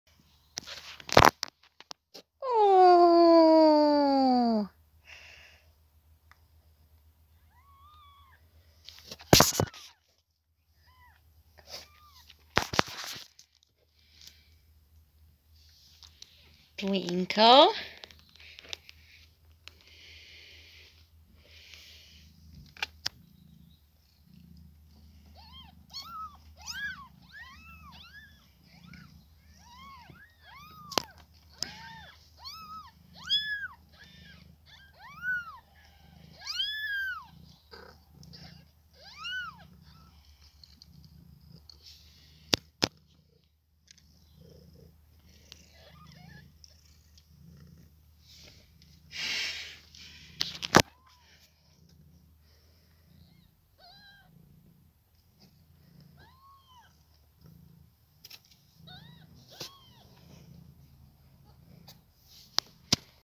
Twinkle's kittens (pure cuteness) 🐱
🐱Description🐱
The kittens were born 9th August 2020. 5 kittens 4 white and tabby (like Twinkle) and 1 black and white. So cute. I don' t know what to name the kittens yet.